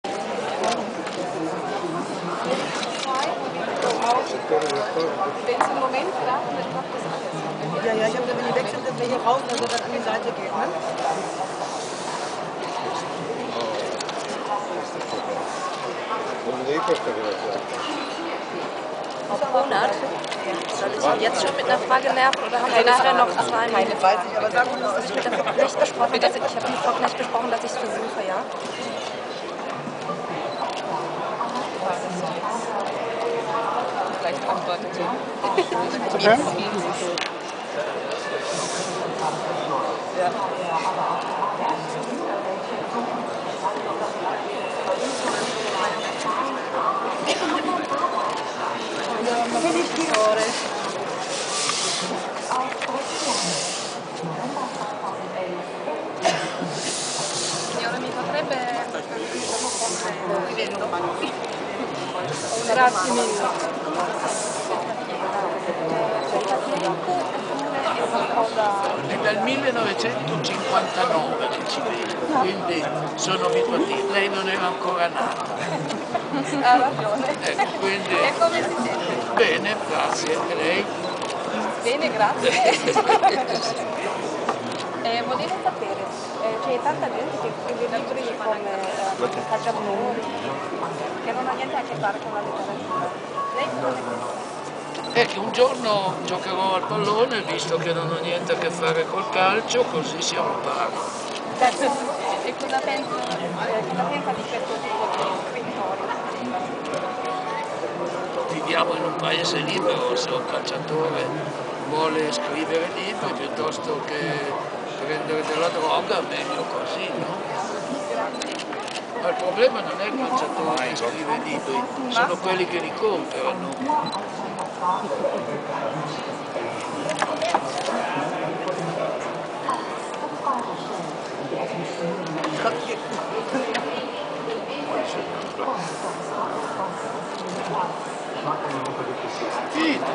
In-Person Autogramme: Umberto Eco schreibt/signed Autogramme/ Autographs 2011
Umberto Eco schreibt/signed Autogramme/Autographs am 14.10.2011 auf der Frankfurter Buchmesse.